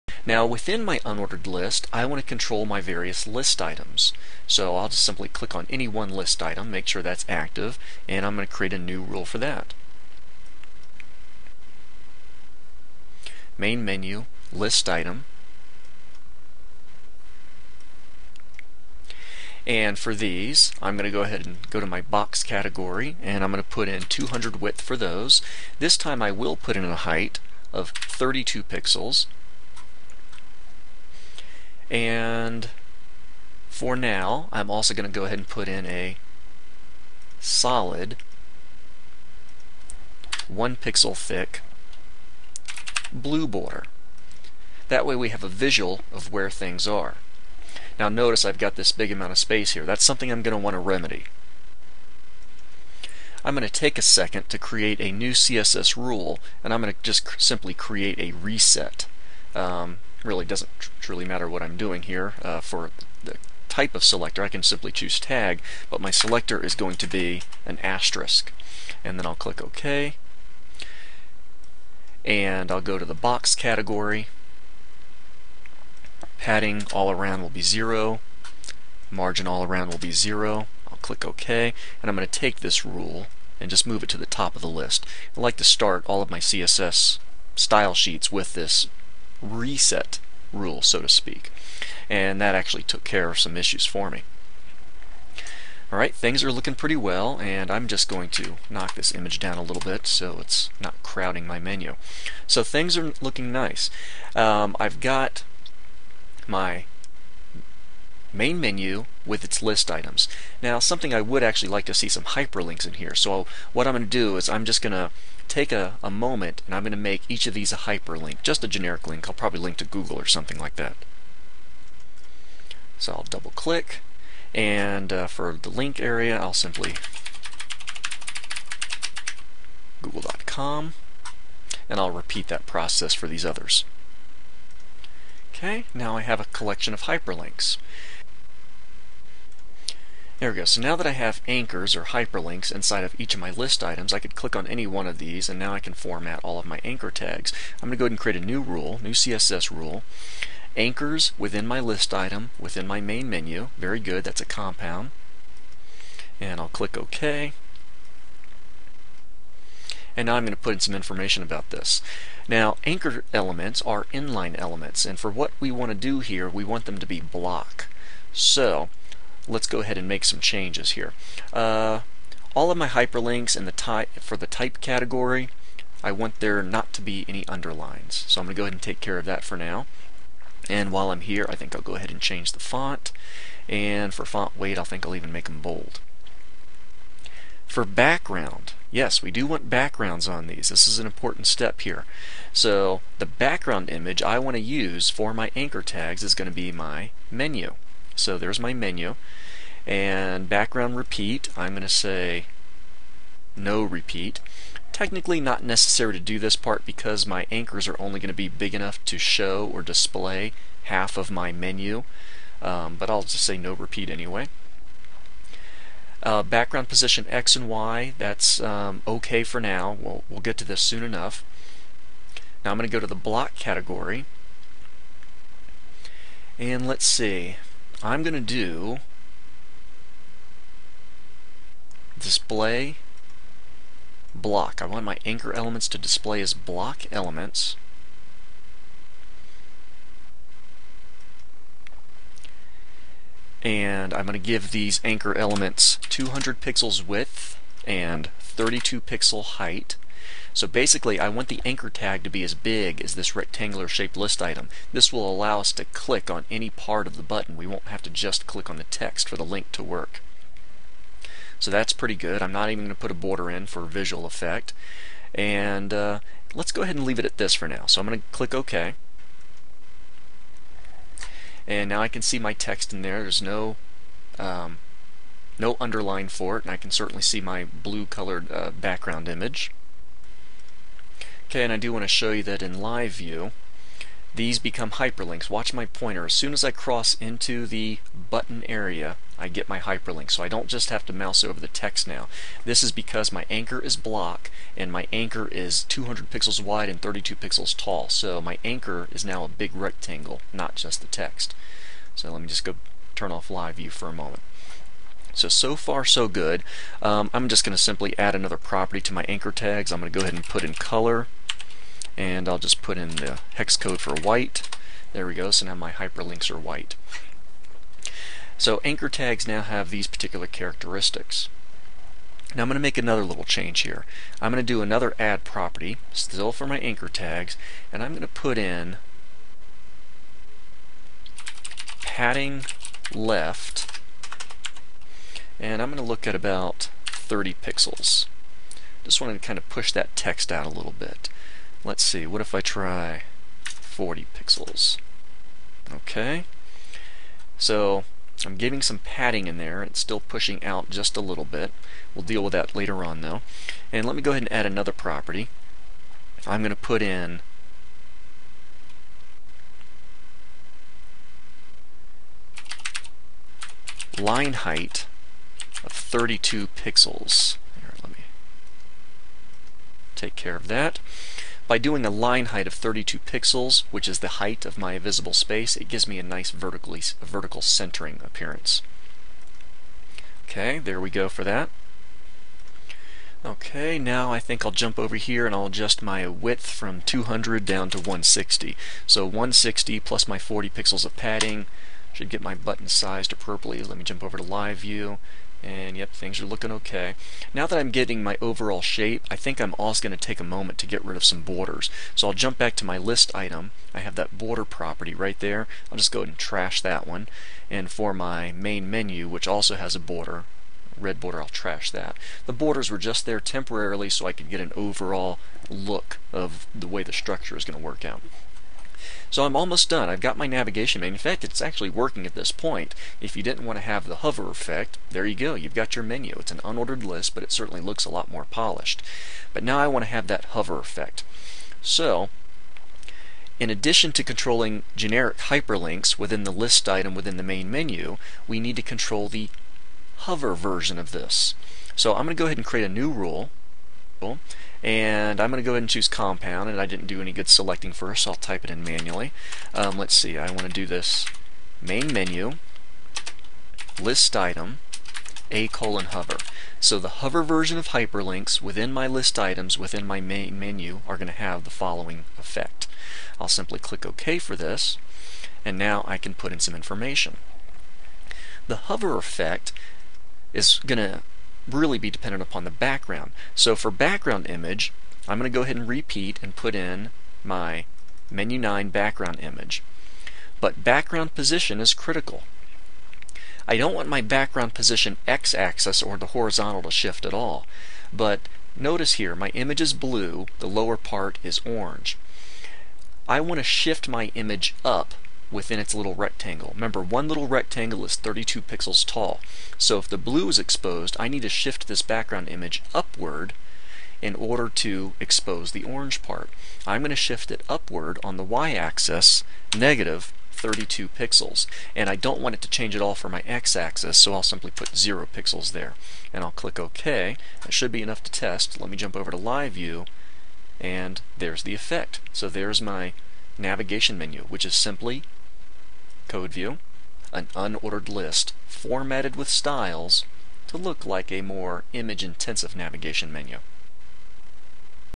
Vertical Navigation Menu in Dreamweaver Part 2
create a vertical navigation menu using an unordered list and a background image that is repositioned on hover.